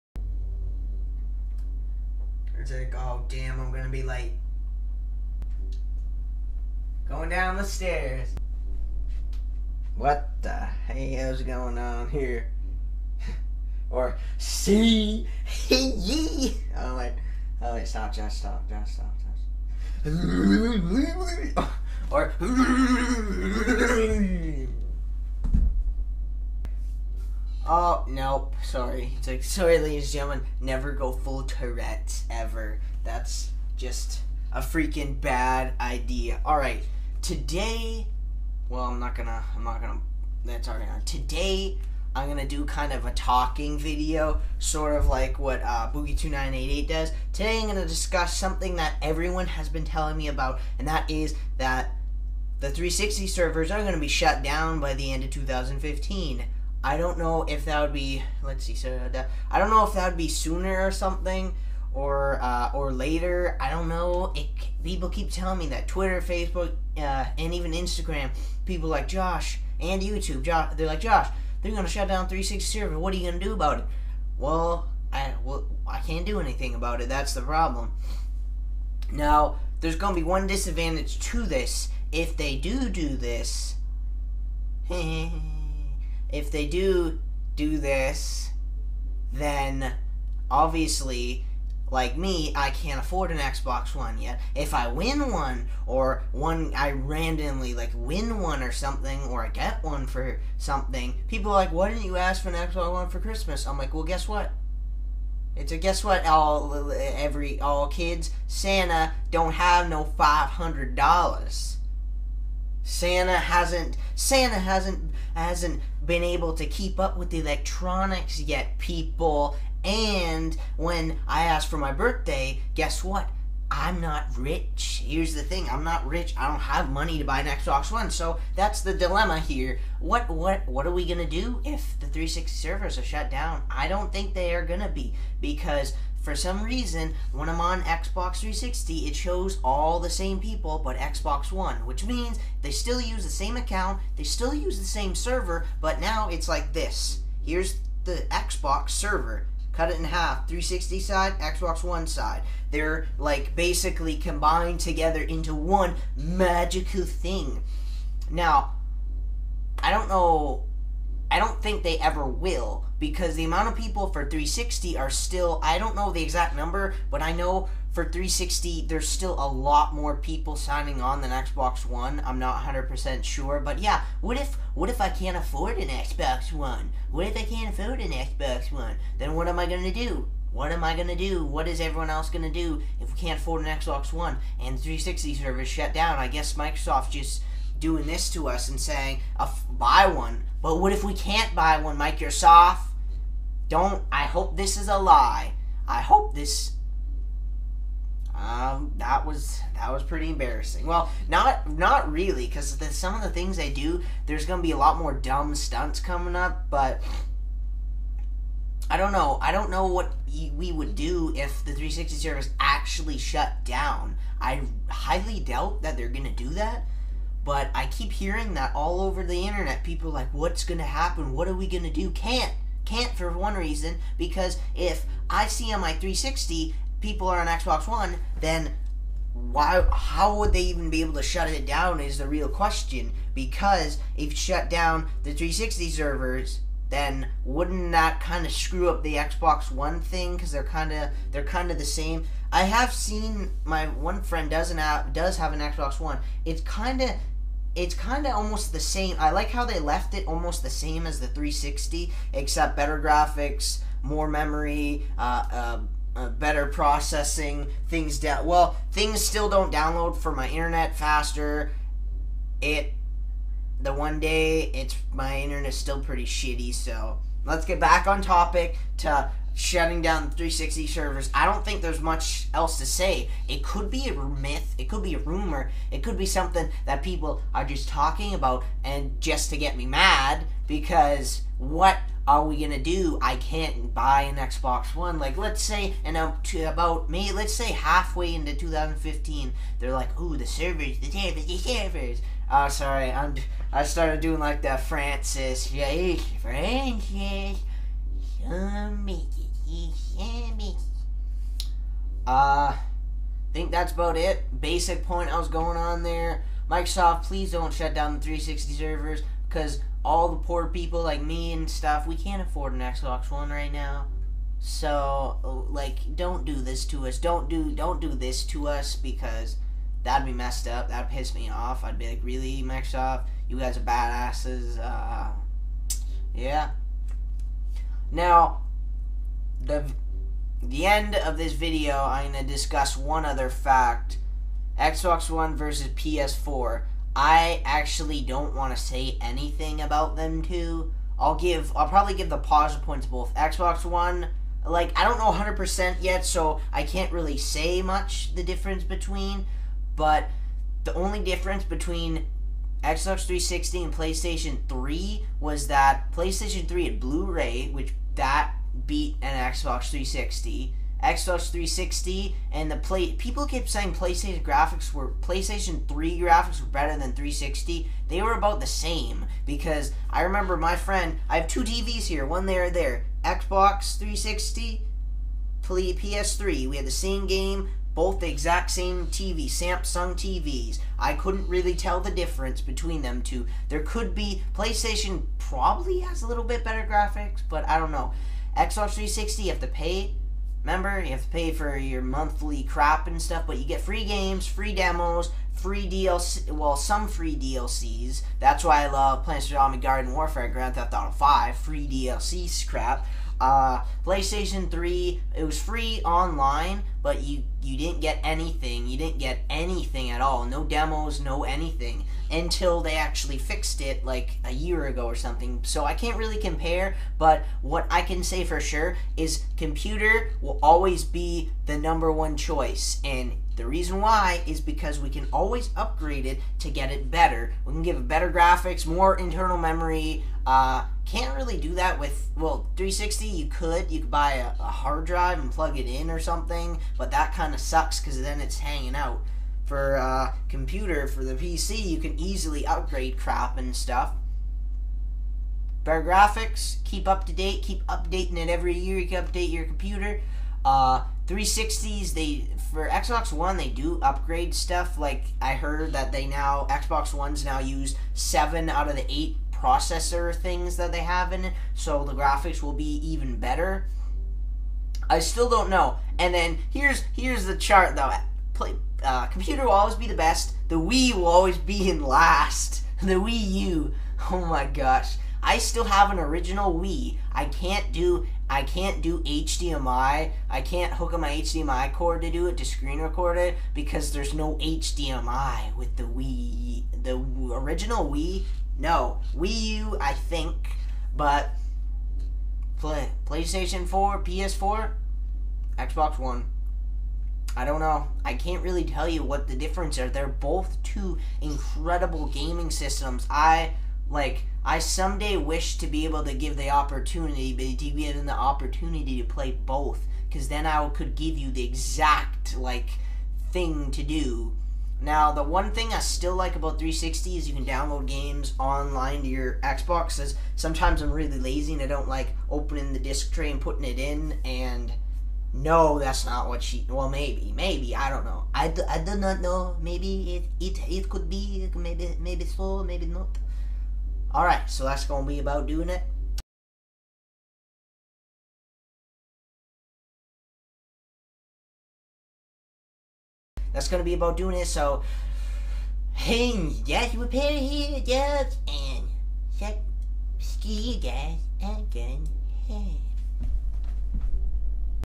Are The Xbox 360 Servers Going To Get Shutdown
in this video i talk about if or when will microsoft will shut down the xbox 360 servers and on the battle between xbox and playstation guess what about after i did this video i found out that xbox 360 servers are going to be shut down in 3 years